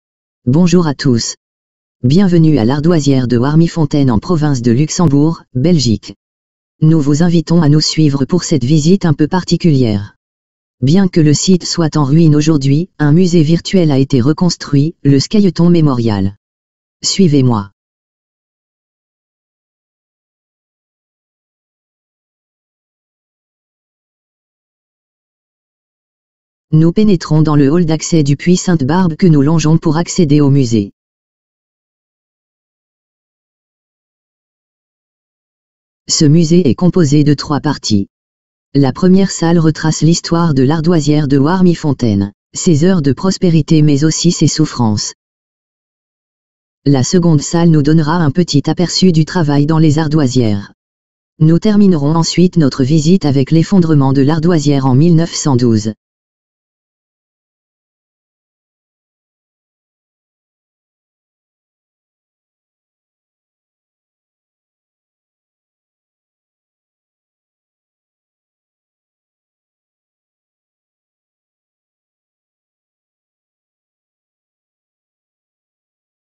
Scailleton Memorial - Introduction générale
Bienvenue dans le Scailleton Memorial, un musée virtuel en mémoire de tous les ouvriers de l'ardoisière de Warmifontaine.
Vidéo interactive où vous pouvez visiter à votre guise les 3 différentes salles de ce musée :
1. L 'ardoisière de Warmifontaine,  éléments principaux
2. Le travail des ardoisiers, .
3. L'effondrement de 1912
Cliquer en fin de chaque vidéo sur l'icône de votre choix pour continuer votre visite.